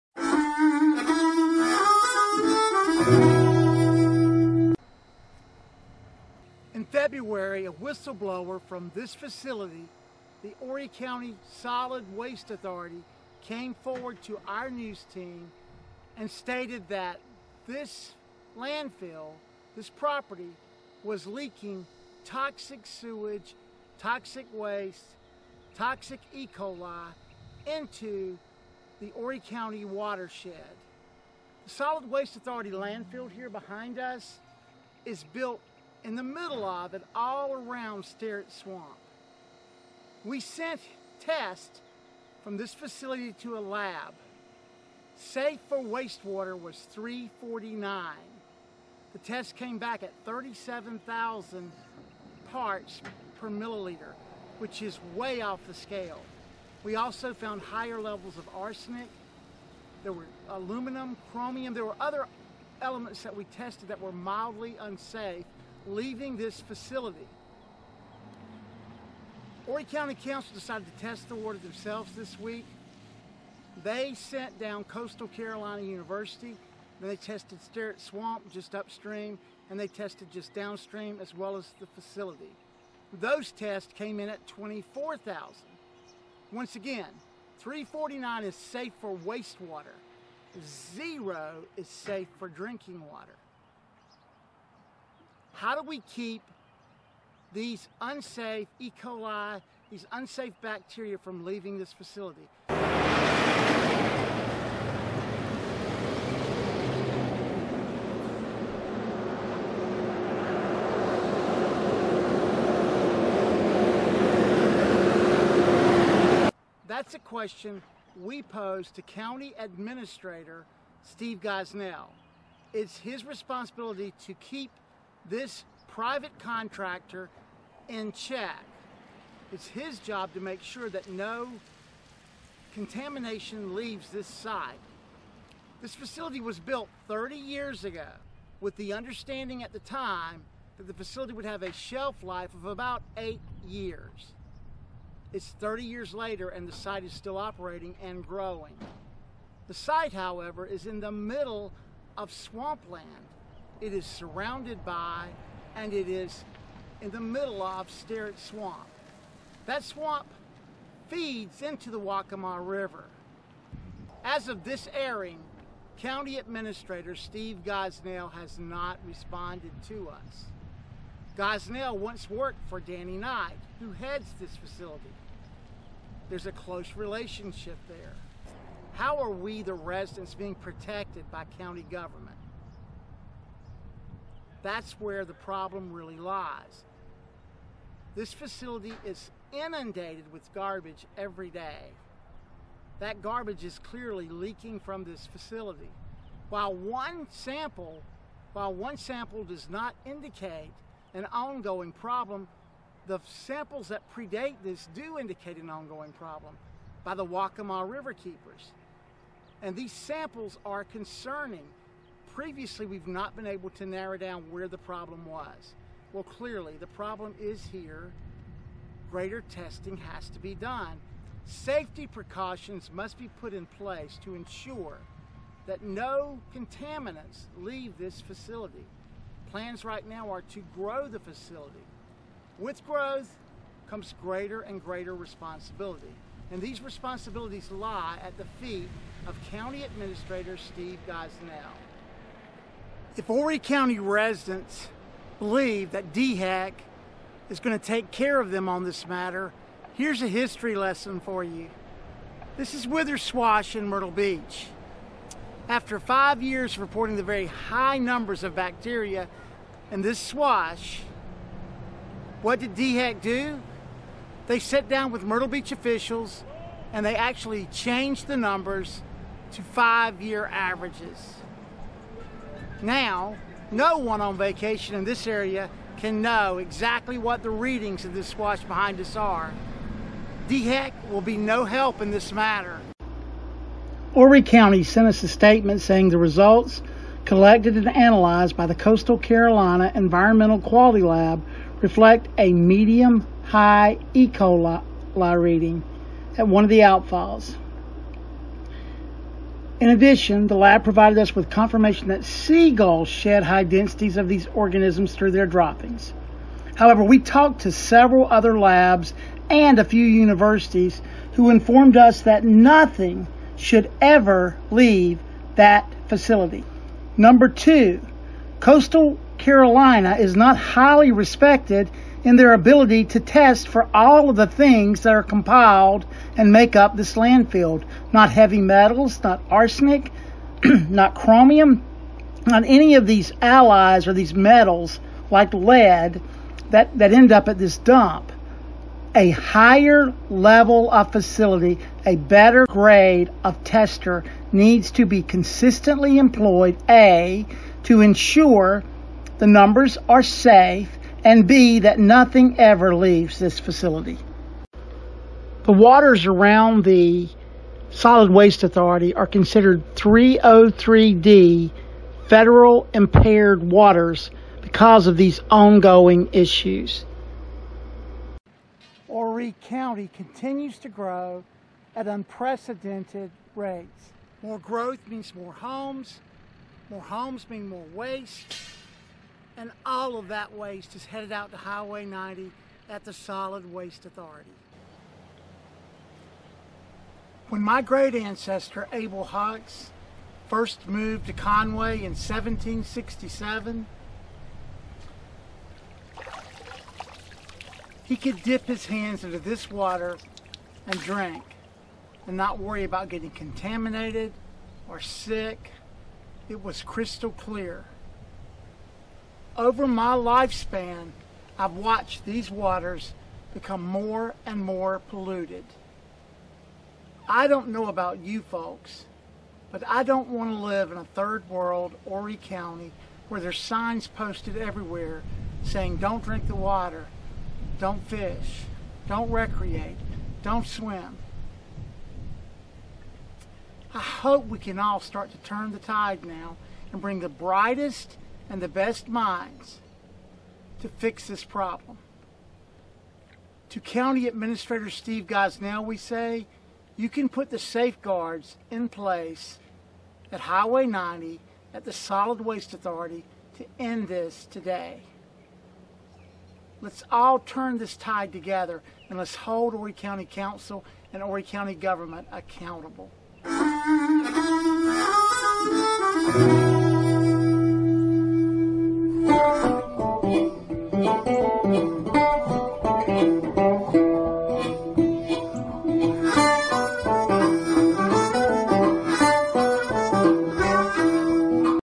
WHISTLE BLOWER - SWA DUMPING DANGEROUS LEVELS OF ECOLI INTO THE HORRY COUNTY WATERSHED
The Solid Waste Authority dumped levels of up to 37,000 ecoli bacteria into the the watershed.  349 is safe for waste water.  0 is safe for drinking water.